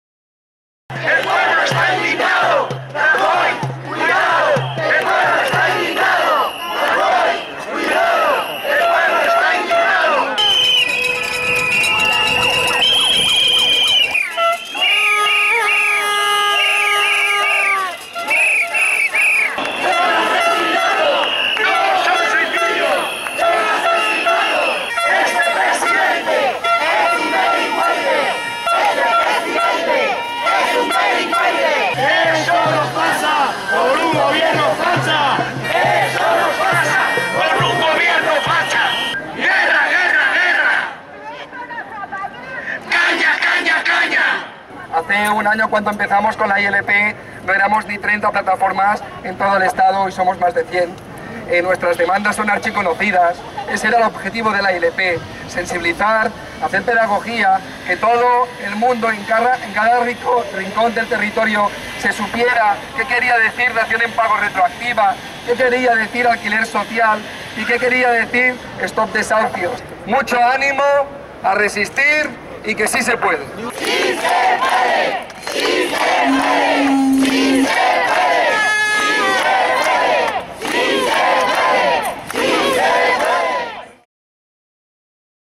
Protesta anti-desahucios frente a la sede del Partido Popular
Concentración en Barcelona frente a la sede del gobernante Partido Popular el 12 de febrero de 2013 para exigir el fin de los desahucios, la dación en pago y el alquiler social en el Estado español. Un reportaje de Oriol Sabata.